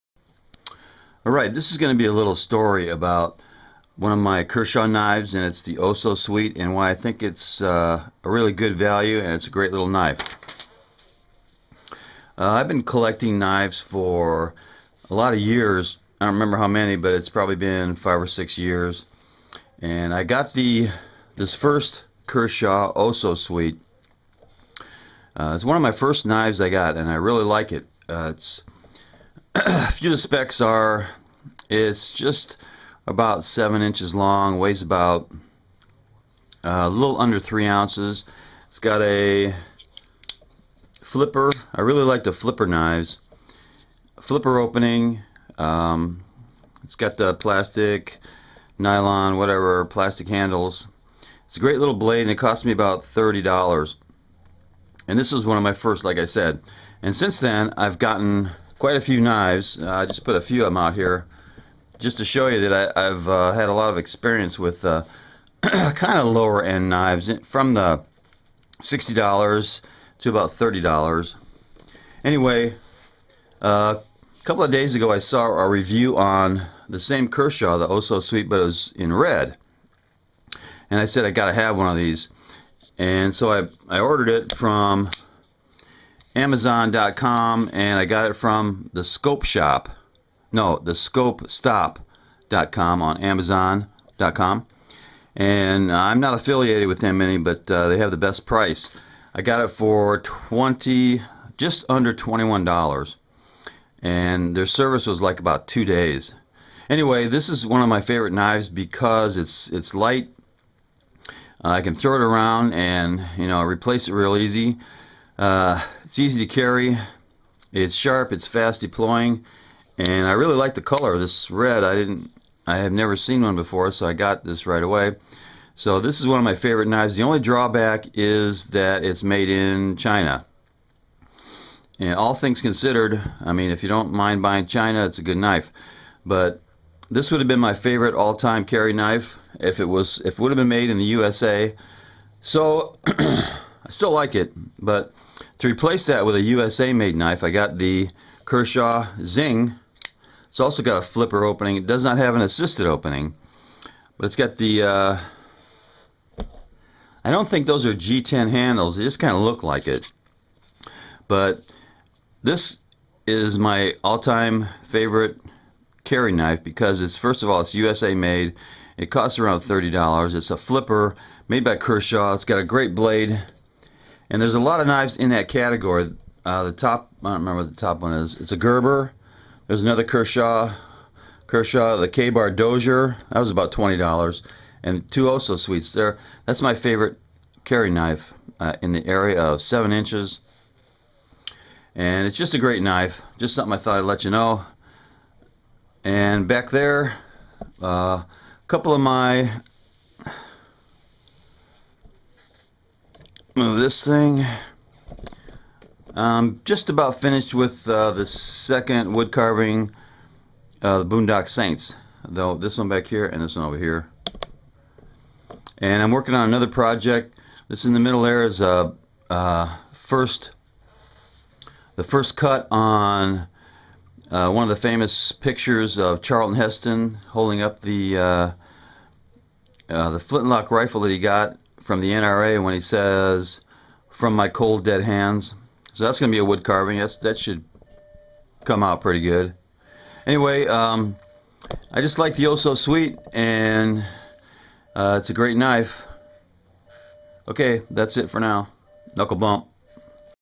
The Kershaw OSO Sweet in Red.
The OSO Sweet is a good value but not made in USA.